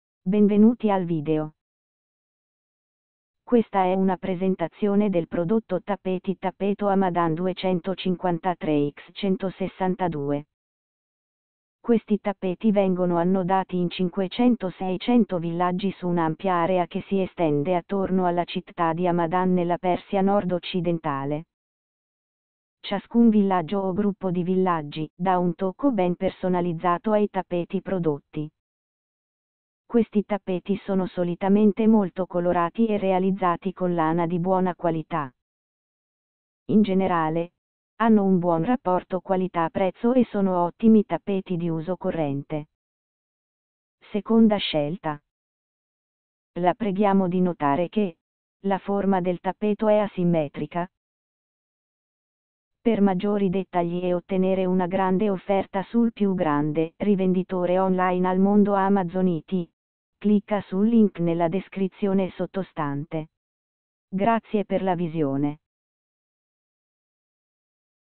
Tappeto Hamadan 253x162
Questi tappeti vengono annodati in 500-600 villaggi su un'ampia area che si estende attorno alla città di Hamadan nella Persia nord-occidentale. Ciascun villaggio o gruppo di villaggi dà un tocco ben personalizzato ai tappeti prodotti. Questi tappeti sono solitamente molto colorati e realizzati con lana di buona qualità. In generale, hanno un buon rapporto qualità-prezzo e sono ottimi tappeti di uso corrente. Seconda scelta . La preghiamo di notare che: La forma del tappeto è asimmetrica